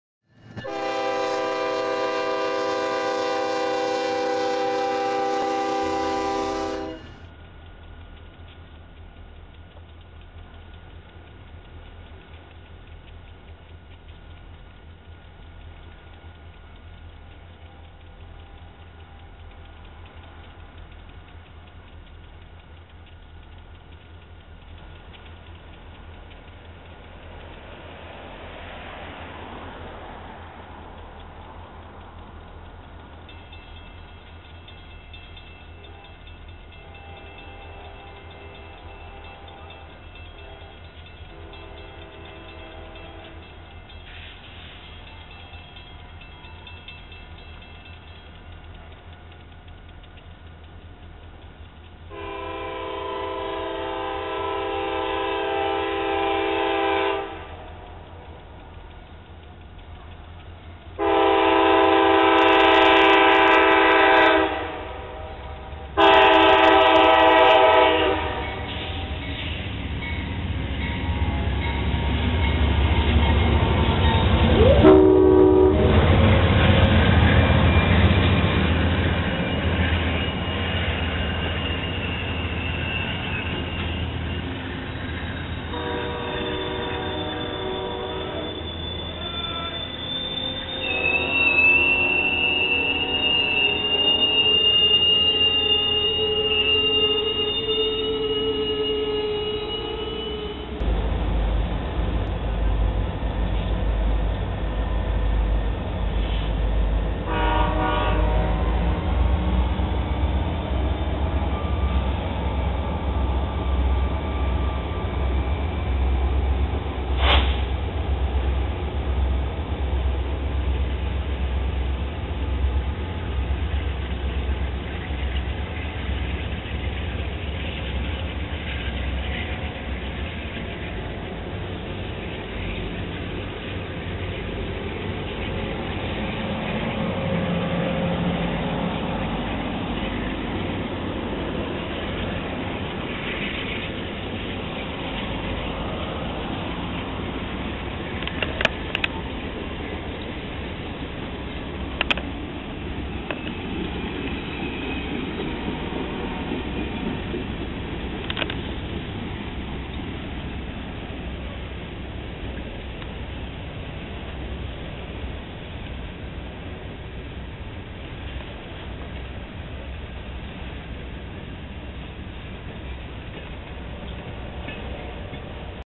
Amtrak 20 with American Orient Express 'Berlin' & New York Central 'Hickory Creek'
Thinking that the Phase III heritage unit was on the point of 20, I went out to the depot to catch it.
No luck on the Heritage unit, but there were two private cars on the rear.
One in the old American Orient Express scheme and the other the classic New York Central 20th Century Limited observation car Hickory Creek.